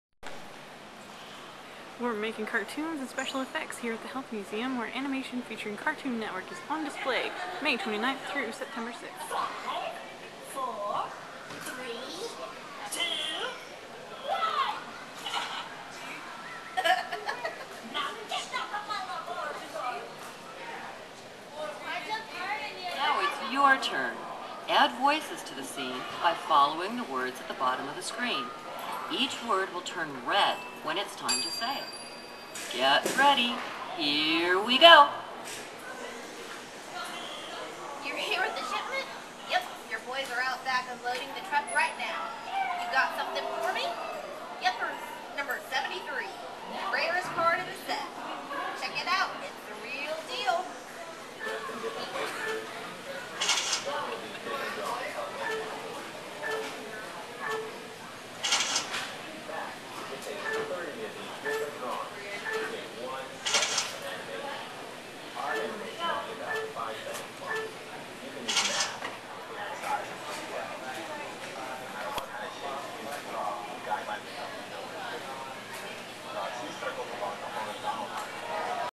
Animation at The Health Museum
Explore how art, math, science, and technology come together in the exciting world of Animation, on display at The Health Museum May 29 - September 6, 2010. Experiment with storyboarding, character design, movement, timing, filming, sound and more!